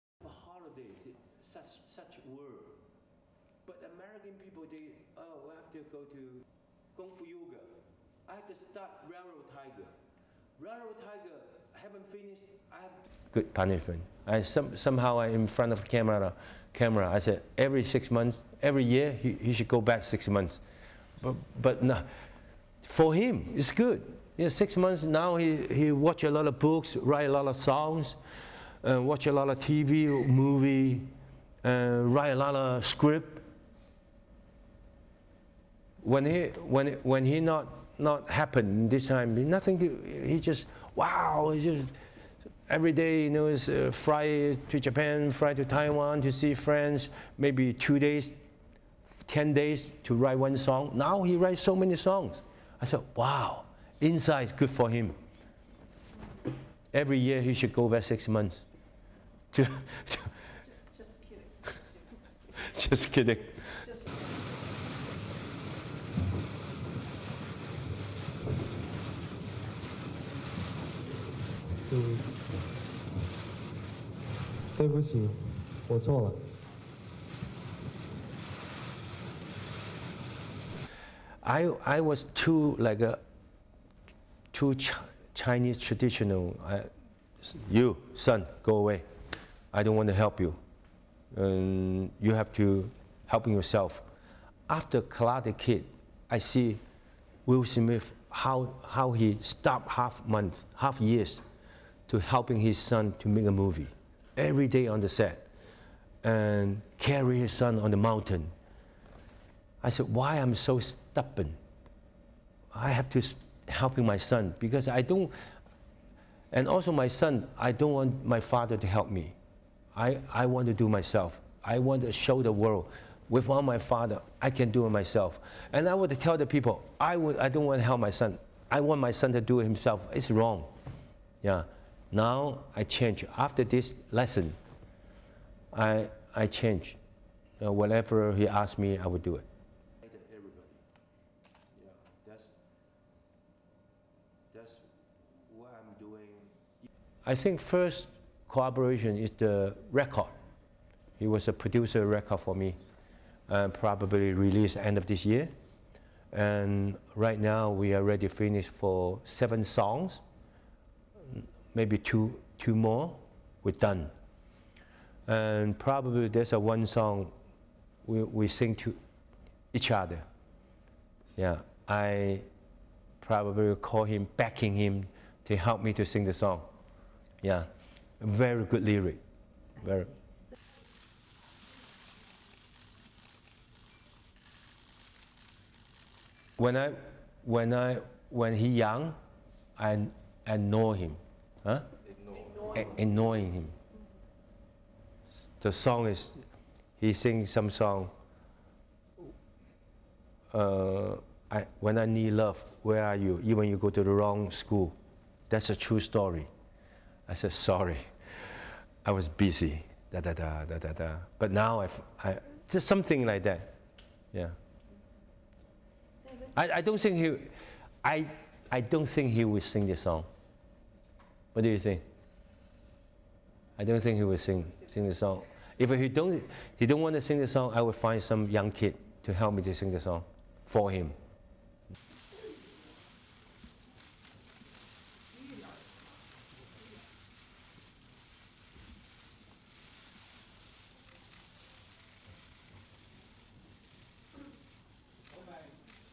Jackie Chan talks candidly about his son Jaycee Chan
(5 Aug 2015) CHAN TALKS CANDIDLY ABOUT ROCKY RELATIONSHIP WITH SON JAYCEE
Things haven't been easy for Jackie Chan and his son Jaycee.
The action star's son - who is also an actor and singer like his father - was released from a six-month prison sentence in February after pleading guilty to allowing others to use marijuana at his Beijing apartment.
Chan, who admits he has a tough love approach to parenting, said Jaycee's incarceration has been "good for him."
"It was good punishment," noted Chan.
"For him, it's good. In the six months, now he reads a lot of books, writes a lot of songs, watches a lot of TV and movies and writes a lot of scripts. Before that happened, everyday he'd just fly to Japan, fly to Taiwan to see friends. Maybe he'd take two days to ten day to write one song. Now he writes so many songs. I thought wow, being inside is good for him. Every year he should go back inside for six months," said Chan, before adding "just kidding."
Father and son are now working on an album as they go about mending their relationship - which Chan said was strained due to his "traditional" ways.
"I was a too traditional Chinese father. Like, 'you, son, go away! I don't want to help you. You have to help yourself,'" recalled Chan.
"After 'The Karate Kid,' and seeing Will Smith stop (his own work) for half a year to help his son to make the movie, every day on the set, carrying his son on the mountain, I said 'why am I so stubborn? I have to help my son.' And also my son, he's like 'I don't want my father to help me. I want to do it myself. I want to show the world without my father that I can do it myself.' And I wanted to tell the people 'I don't want to help my son. I want my son to do it himself.' It's wrong. Now I've changed," explained Chan.
"After this lesson I changed. Now whatever he asks me, I will do it."
Chan is now looking for suitable movie projects for 32-year-old Jaycee and, in the meantime, his son is set to produce one of his records, expected out at the end of the year.
Chan's also hoping Jaycee will sing a duet with him on one of the album tracks.
"Probably there's one song we will sing with each other. I will probably call him begging him to help me sing the song. It's got very good lyrics," said Chan.
"When he was young, I ignored him. He sings in the song: 'when I need love where are you, even you go to the wrong school.' That's a true story. I said 'I'm sorry, I was busy dadadada'�but now�something like that. I don't think he will sing that song. What do you think? I don't think he will sing the song. If he doesn't want to sing the song, I will find some young kid to help me sing the song, for him."
After the detention of Jaycee and other actors on drugs charges, Chinese authorities said they would ban celebrities linked to drugs from appearing in movies and TV shows in China.
However, the younger Chan still appeared in a small role in Chen Kaige's recent "Monk Comes Down the Mountain," which had already finished filming when Chan was detained.